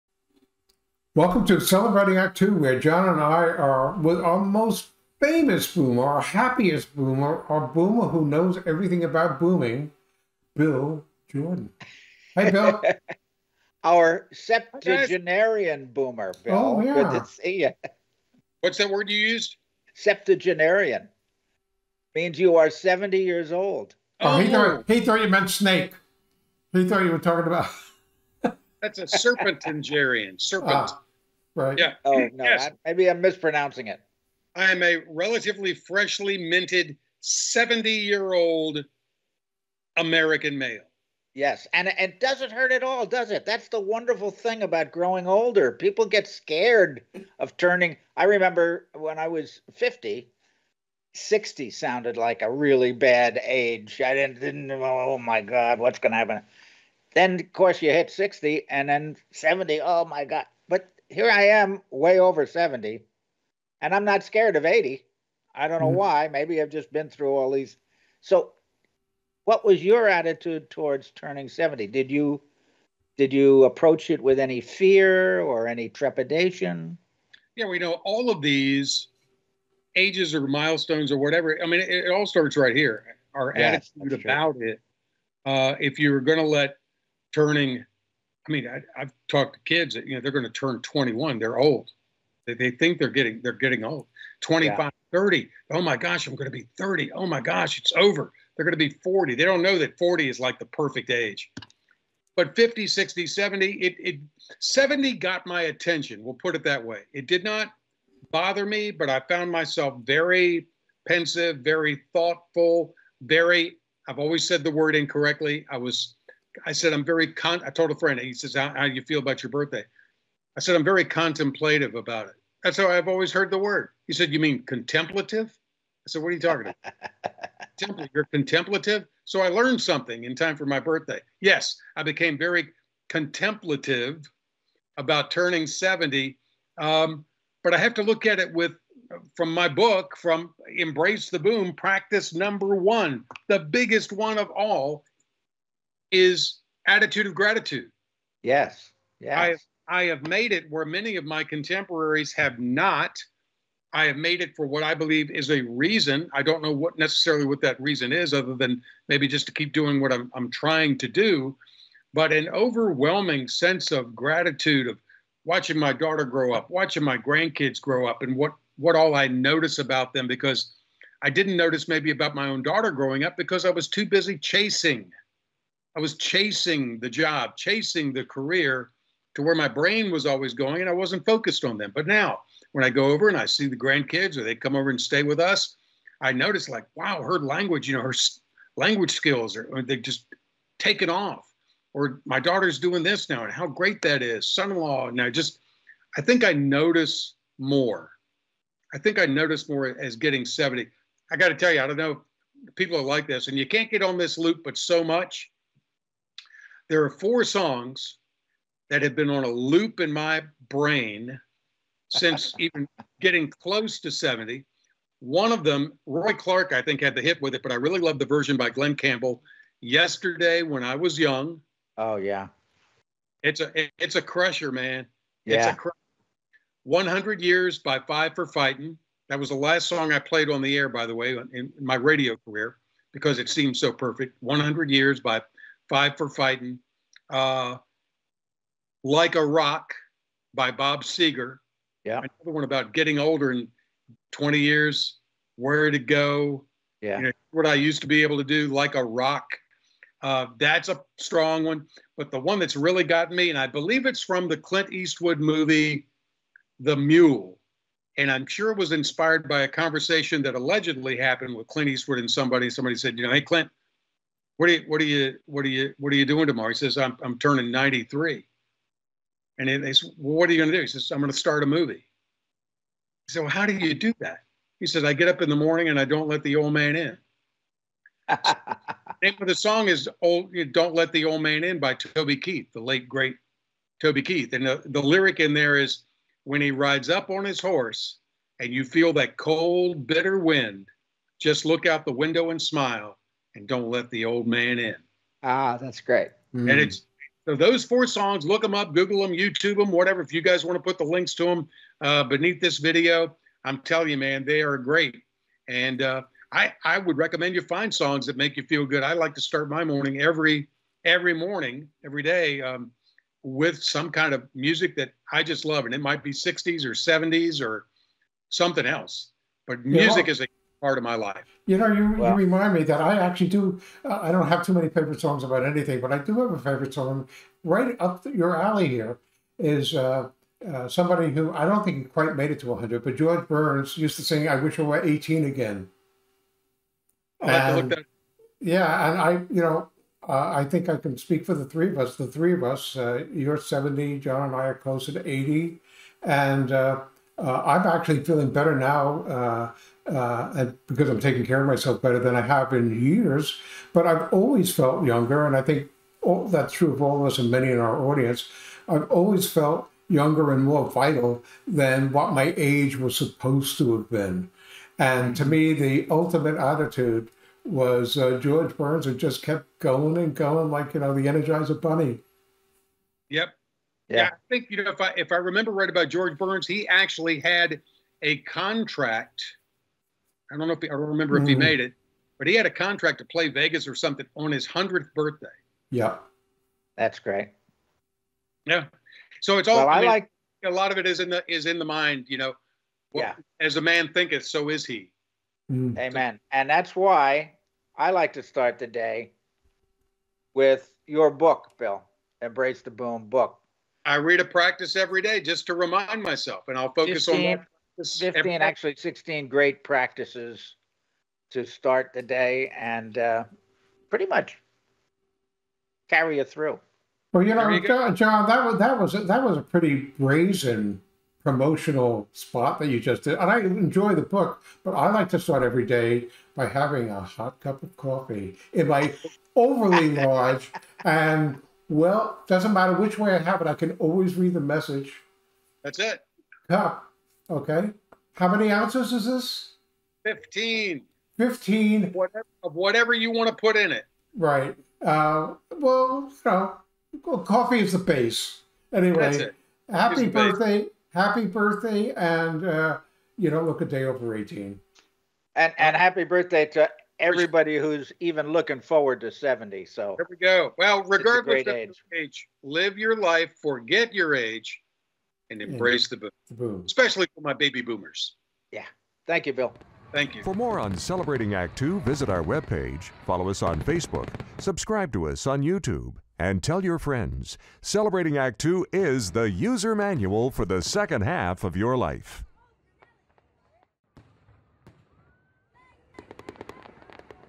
Bill Jordan: Is Being Old Just in Your Head?
So he's turned 70 recently --but refuses to let that define him! how about you? Is your age a reality or just a construct in your head holding you back?

For more on Bill Jordan and to order his mug go